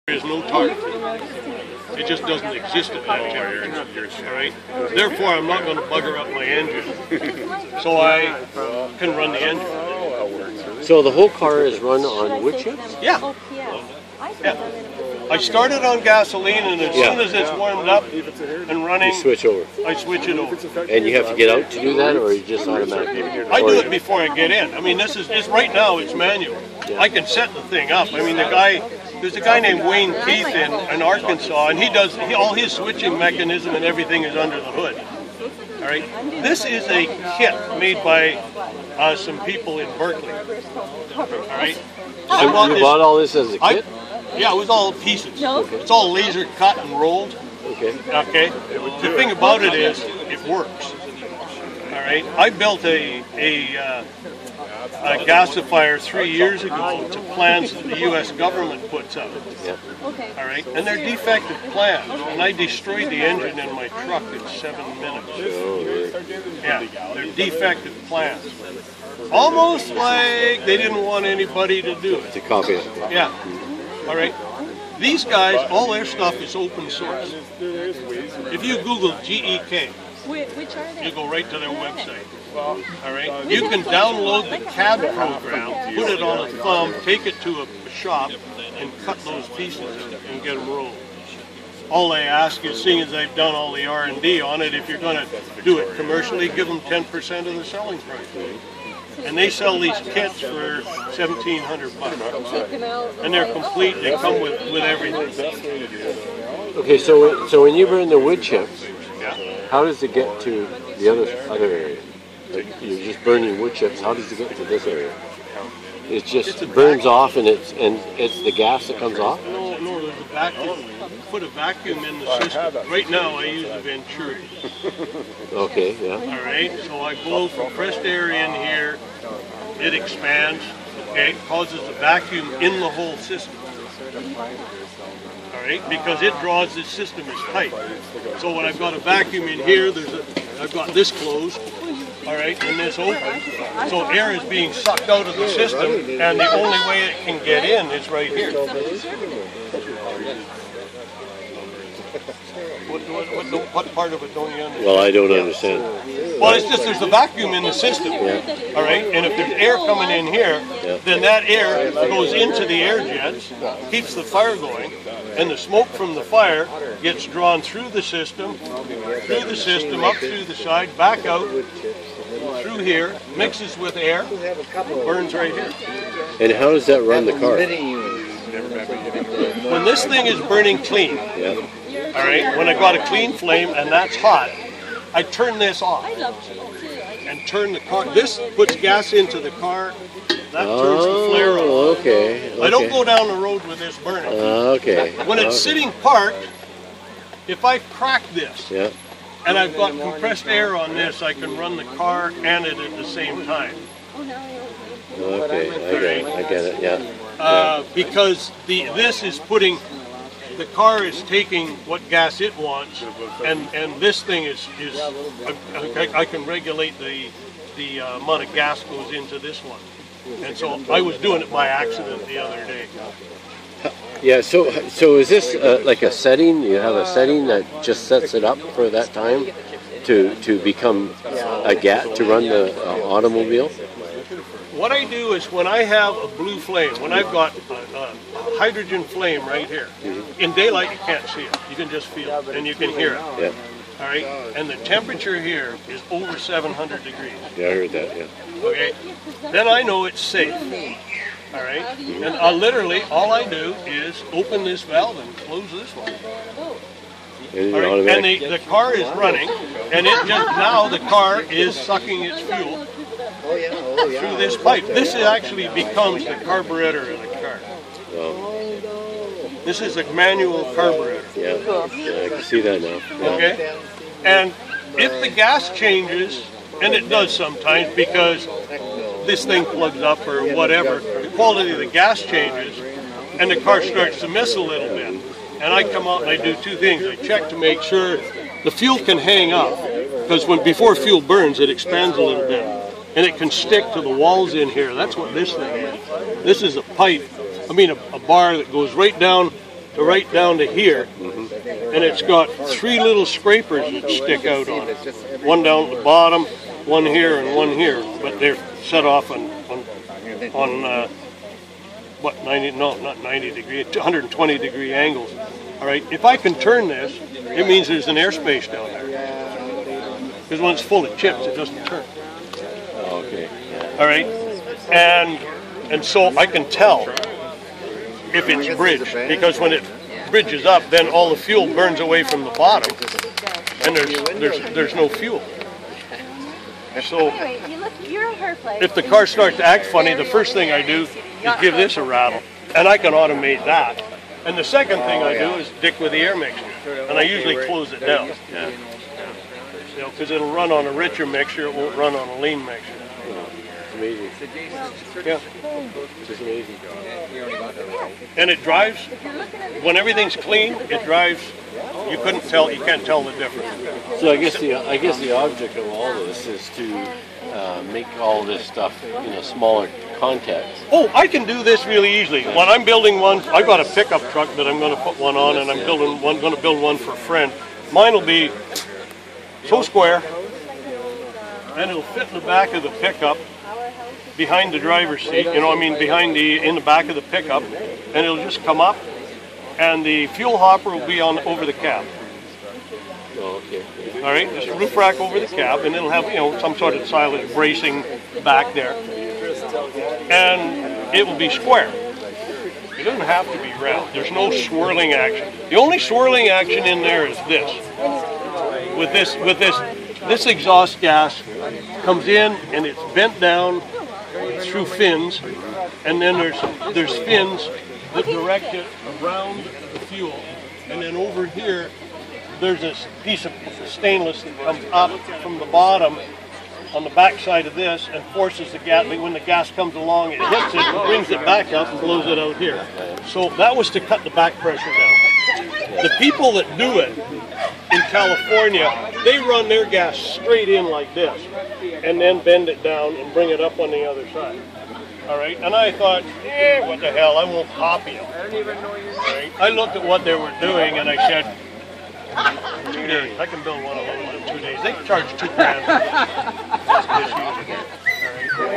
Wood Burning Gas Car - Runs on Burning Wood Chips
Found this guy on Lasqueti Island (British Columbia, Canada) who has converted a car to run on wood chips. The wood burns to create a gas which is then fed into the gas system of the car. Here he is explaining how it works. All the plans are available online for free.